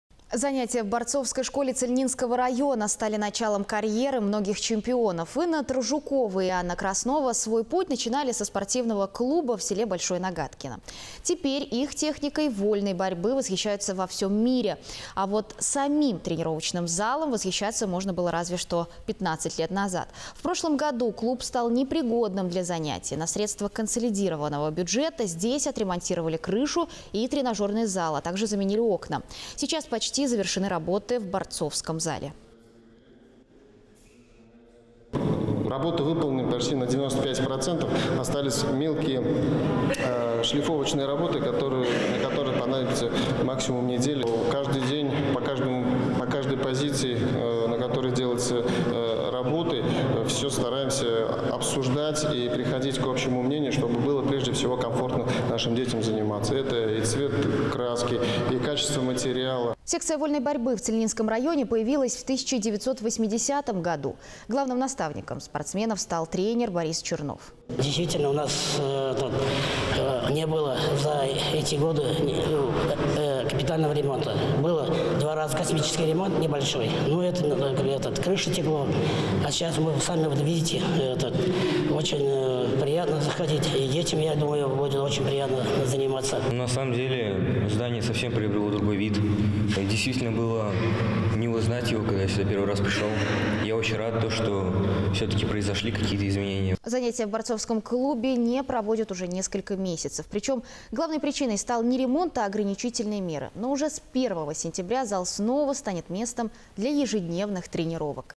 Занятия в борцовской школе
Занятия в борцовской школе Цильнинского района стали началом карьеры многих чемпионов. Инна Тражукова и Анна Краснова свой путь начинали со спортивного клуба в селе Большое Нагаткино.